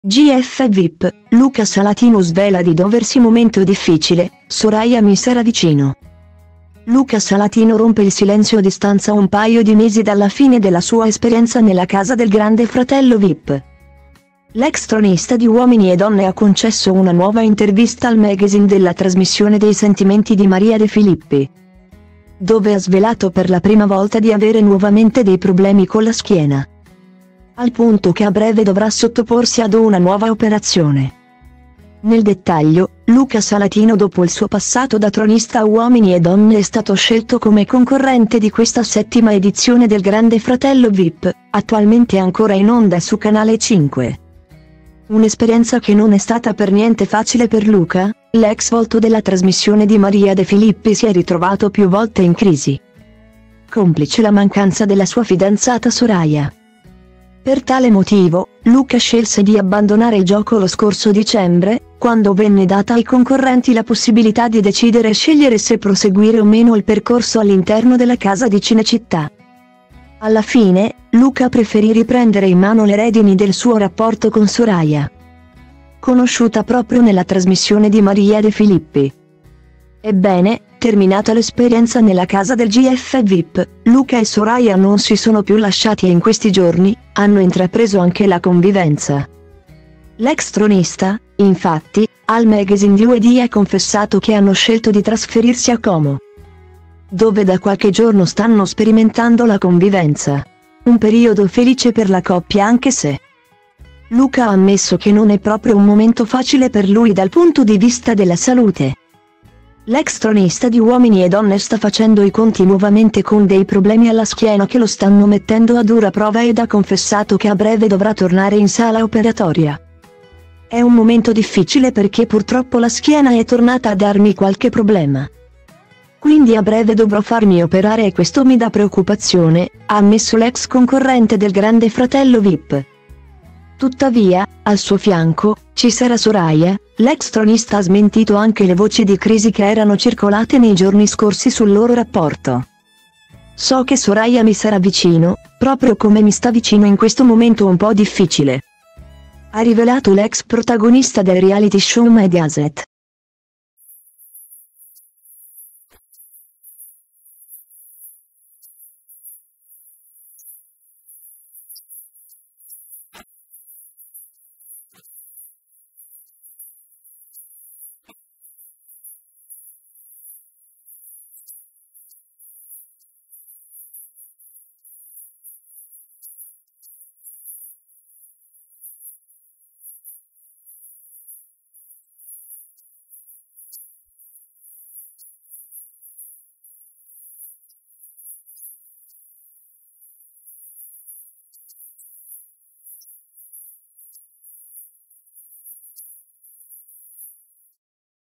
GF Vip, Luca Salatino svela di doversi 'Momento difficile, Soraia mi sarà vicino'
Luca Salatino rompe il silenzio a distanza un paio di mesi dalla fine della sua esperienza nella casa del Grande Fratello Vip. L'ex tronista di Uomini e donne ha concesso una nuova intervista al magazine della trasmissione dei sentimenti di Maria De Filippi,dove ha svelato per la prima volta di avere nuovamente dei problemi con la schiena,